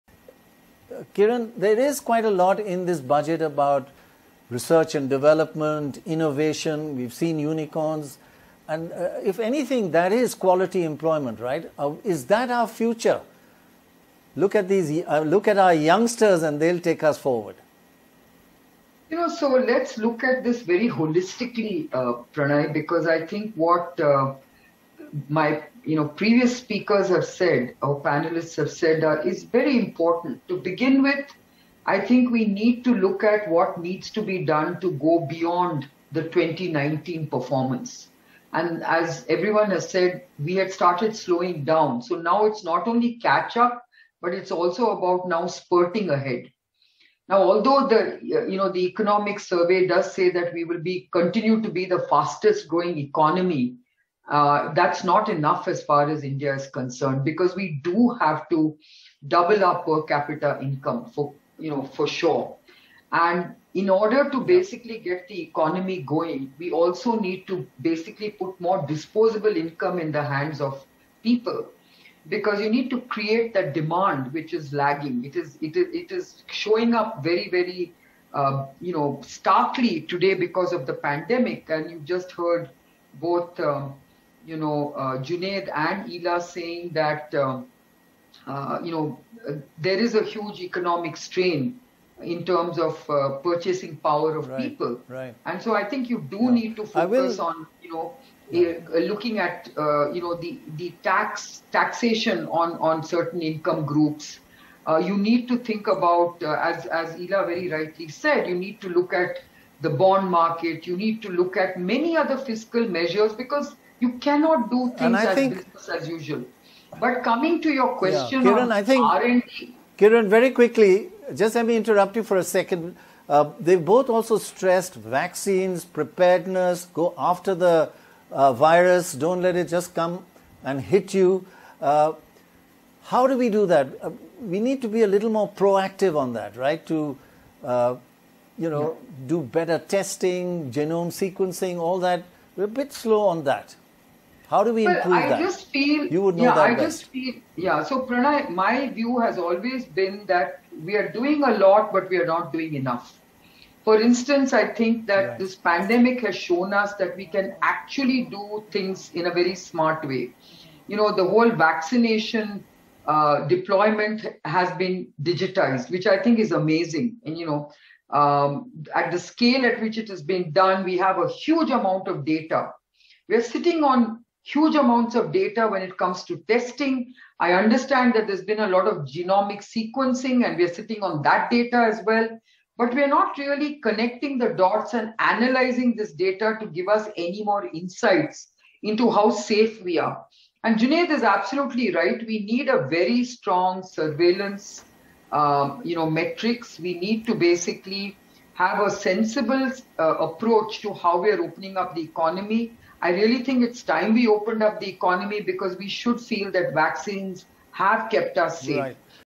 Must Double Up Per Capita Income: Kiran Mazumdar Shaw
"Although the economic survey does say that we will continue to be the fastest growing economy, it is not enough as far as India is concerned. We do have to double up per capita income for sure," says Kiran Mazumdar Shaw, Executive Chairperson Biocon Limited.